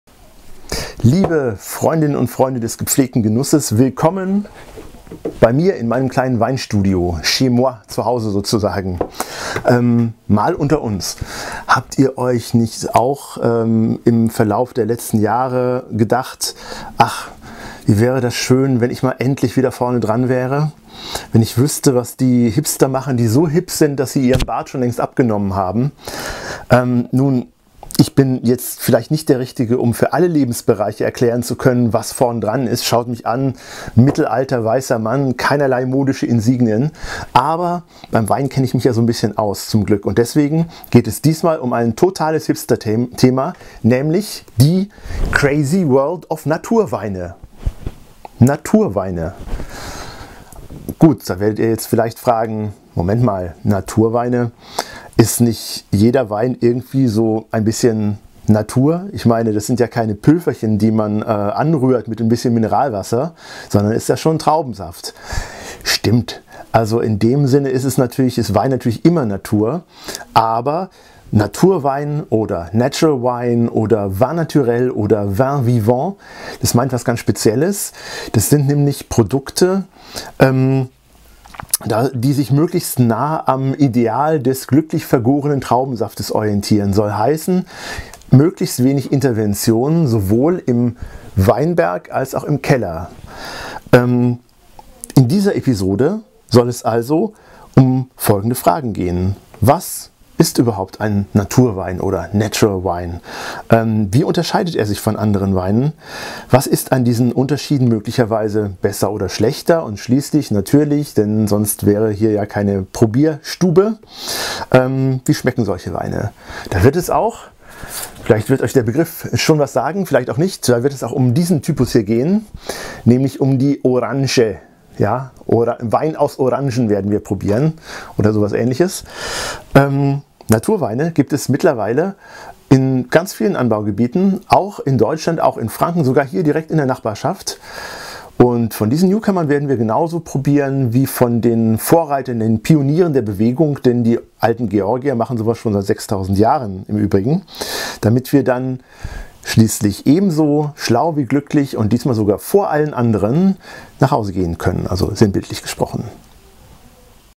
Chez Matze Weinkurs - Die crazy world of Naturweine
Was soll denn bitteschön ein "Naturwein", ein "Natural Wine" sein? Und wie unterscheidet er sich von anderen Weinen? Ein Super-Hipster-Thema, unpassend angekündigt im Karohemd.